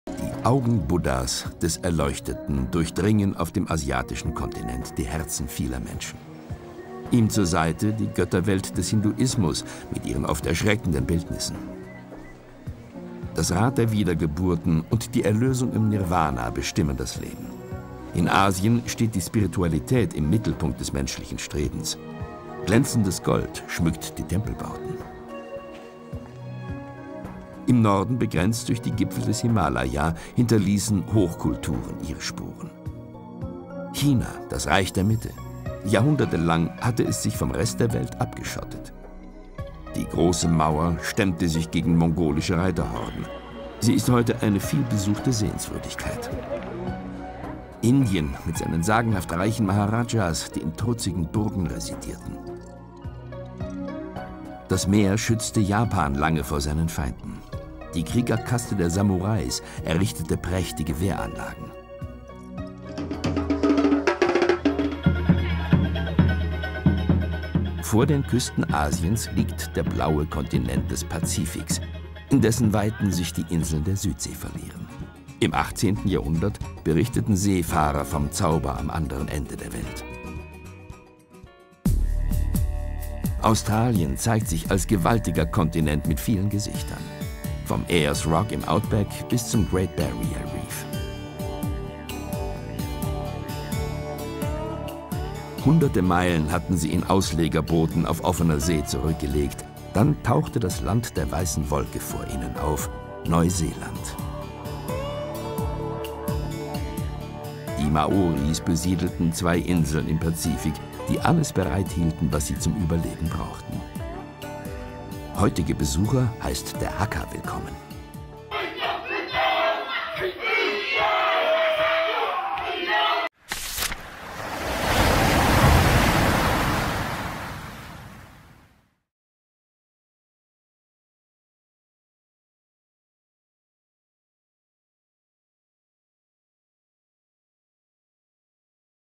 Asien und Pazifik: Highlights - Reisebericht
Asien - der größte Kontinent unseres Planeten. Ein Schmelztiegel der Völker und Glaubenslehren. Vier Milliarden Menschen leben in den 47 Ländern auf ihre eigene Weise, ganz anders als in Europa.
Von der chinesischen Mauer bis zur Shwedagon-Pagode im wuseligen Yangon, der Hauptstadt Burmas, zeigt FaszinationFerne die Highlights dieses kontrastreichen Kontinents.
Abgerundet wird der Streifzug durch einen Ausflug in den Pazifik, nach Australien und Neuseeland.